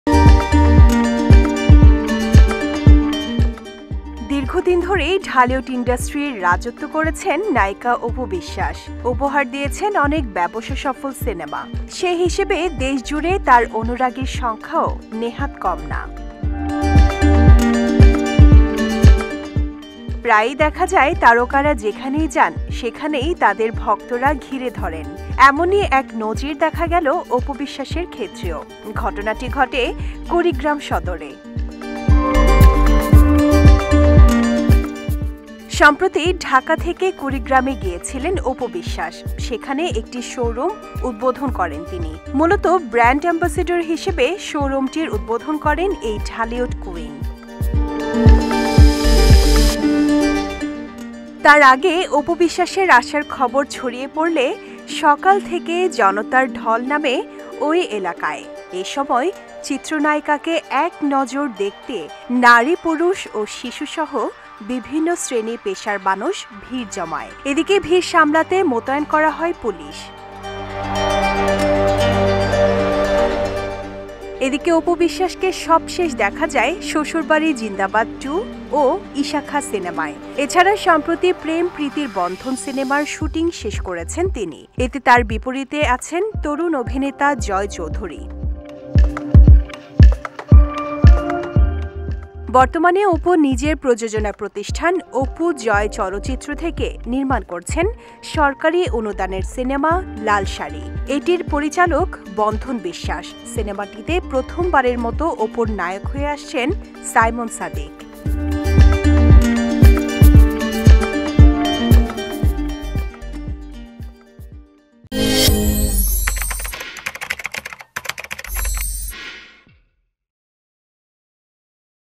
অপু বিশ্বাসকে দেখতে জনতার ভিড়। Apu Biswas। Bijoy TV
দীর্ঘদিন ধরেই ঢালিউড ইন্ডাস্ট্রি রাজত্ব করেছেন নায়িকা অপু বিশ্বাস। উপহার দিয়েছেন অনেক ব্যবসাসফল সিনেমা। সে হিসেবে দেশজুড়ে তার অনুরাগীর সংখ্যাও নেহাত কম না।
প্রায়ই দেখা যায় তারকারা যেখানেই যান সেখানেই তাদের ভক্তরা ঘিরে ধরেন। এমনই এক নজির দেখা গেলো অপুর বিশ্বাসের ক্ষেত্রেও। ঘটনাটি ঘটে কুড়িগ্রাম সদরে।
সম্প্রতি, ঢাকা থেকে কুড়িগ্রামে গিয়েছিলেন অপু বিশ্বাস। সেখানে একটি শোরুম উদ্বোধন করেন তিনি। মূলত ব্র্যান্ড অ্যাম্বাসেডর হিসেবে শো-রুমটির উদ্বোধন করেন এই ‘ঢালিউড কুইন’।
তার আগে অপু বিশ্বাসের আসার খবর ছড়িয়ে পড়লে সকাল থেকে জনতার ঢল নামে ওই এলাকায়। এ সময় চিত্রনায়িকাকে এক নজর দেখতে নারী-পুরুষ ও শিশুসহ বিভিন্ন শ্রেণি পেশার মানুষ ভিড় জমায়। এদিকে, ভিড় সামলাতে মোতায়েন করা হয় পুলিশ।